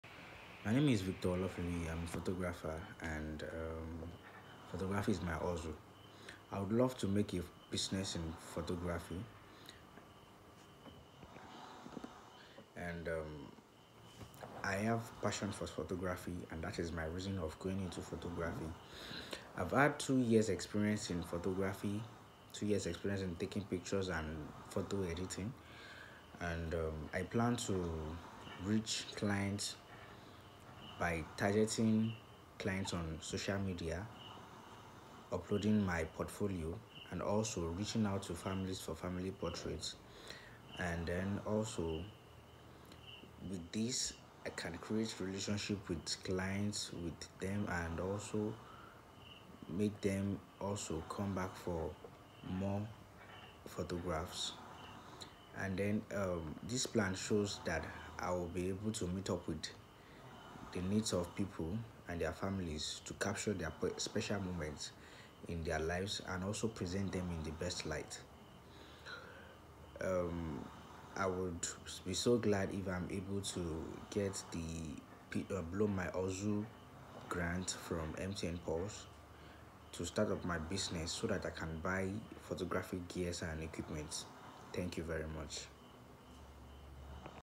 Victor Olofinluyi |Photography
"I am a photographer who wants to make a business in photography. I have had experience in the field of photography and want to pursue a photography business. I have two years experience with taking photos and capturing special moments of people's lives.
I decided to go into the business of photography because of my passion for photography and photo editing. I believe that my business will succeed because I have what it takes, the skill and experience.
I would be very glad if I'm able to get the ""MTN pulse Blow my Hustle"" start up my business so that I can buy necessary equipment and gears.
The growth of the business will be on targeting clients on social media and also reaching out to potential clients and thereby creating relationships with them for ongoing photographs.
This plan shows how I will meet the needs of people and their families to capture special moments in their lives and present them in the best light."mtnpulseblowmyhustle